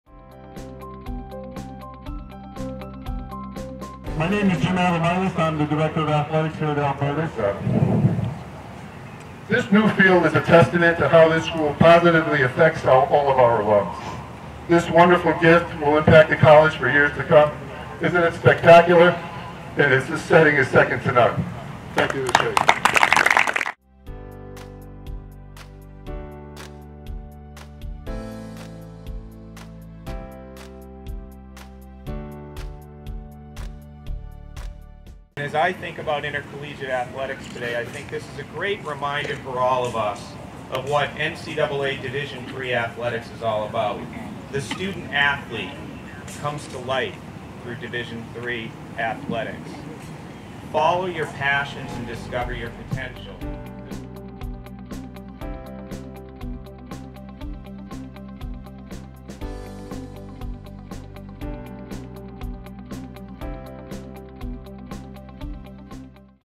Albertus Magnus College Shea Softball Complex Ribbon Cutting
Catch Falcon Fever! Students, faculty members, and family came together to see the official opening, dedication, and ribbon cutting of the new Shea Softball Complex. They also enjoyed the game between Albertus Magnus and Saint Joseph's (Conn.).